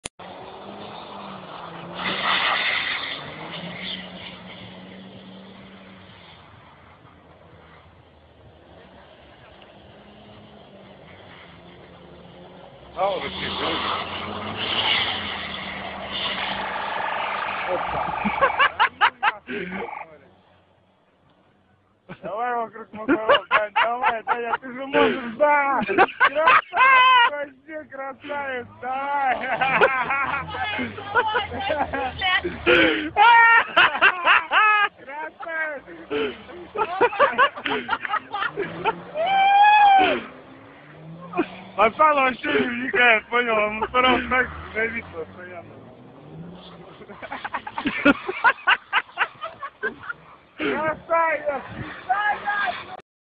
Drift on a frozen parking lot and an unexpected guest
Drift on a frozen parking lot and an unexpected guest as a police car.